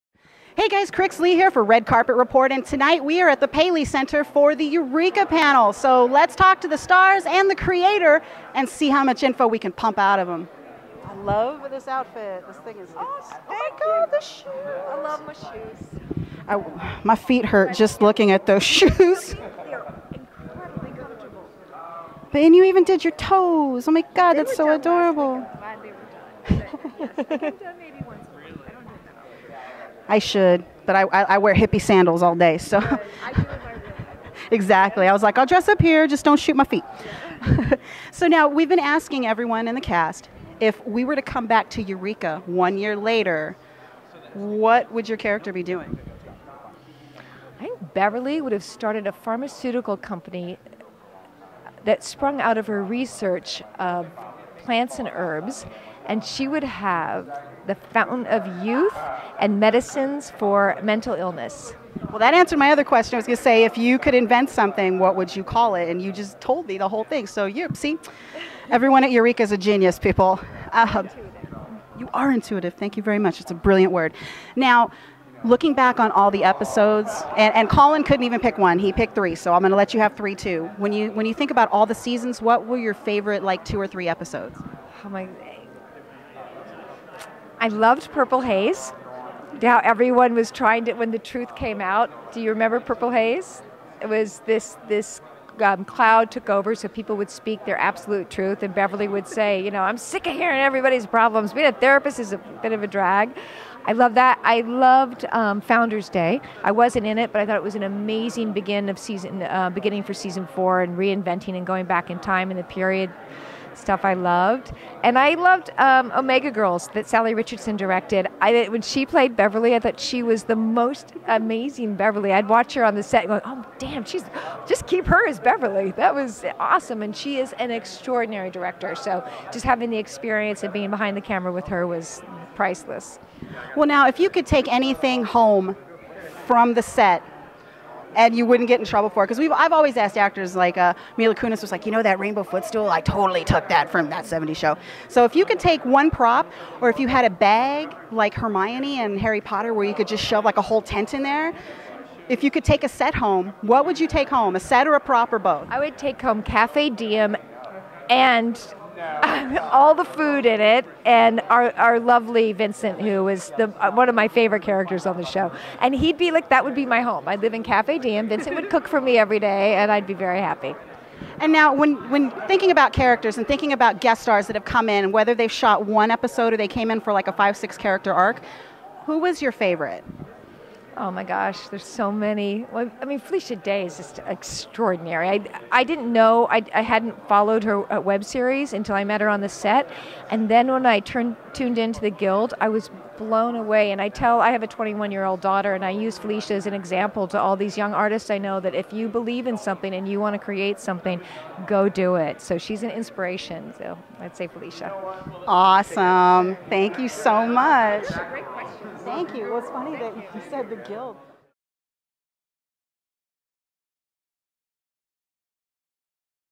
Debrah Farentino at Paley Center's Evening with Syfy's Eureka
Mingle Media TV and Red Carpet Report host, Cricket Lee, were invited to The Paley Center for Media in Beverly Hills for an Evening with Syfy's "Eureka" including a screening, sneak peek of the series finale and followed by a conversation with the show's cast and creative team.

More About Syfy's "Eureka"
Eureka has captivated audiences with its quirky take on a small town with big secrets as it's thrilling fifth and final season prepares to signoff. Eureka stars Colin Ferguson (Sheriff Jack Carter) Salli Richardson-Whitfield (Allison Blake), Joe Morton (Henry Deacon), Erica Cerra (Jo Lupo), Neil Grayston (Douglas Fargo) and Niall Matter (Zane Donovan). Returning for the final season are Wil Wheaton (The Big Bang Theory, Star Trek: The Next Generation) as the contentious Dr. Parrish and Felicia Day (The Guild, Red) as Dr. Holly Marten. Co-creator Jaime Paglia and Bruce Miller are executive producers. The series is produced and distributed by Universal Cable Productions.